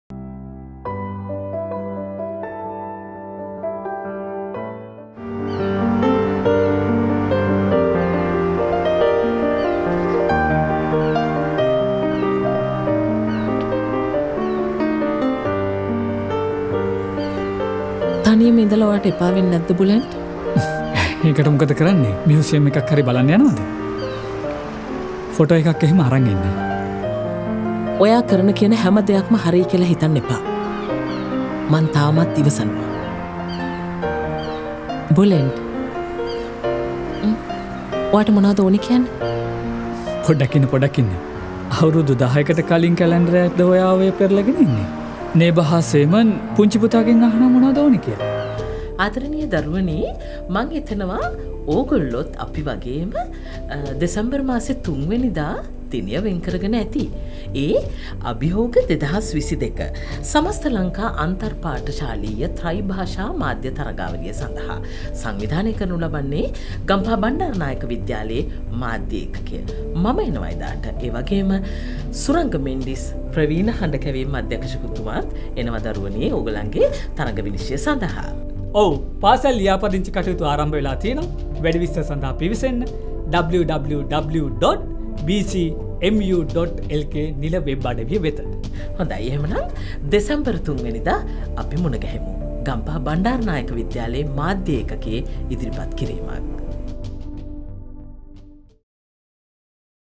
ABHIYOGA '22 - Words from Renowned Panel Of Judges...
ABHIYOGA '22 All Island Inter School Tri-Lingual Media Competition
Words from Renowned Panel Of Judges...
Senior Dubbing Artist Udani Rathnayake & Veteran Dubbing Director Suranga Mendis
The Registration Deadline - 1st Of December 2022...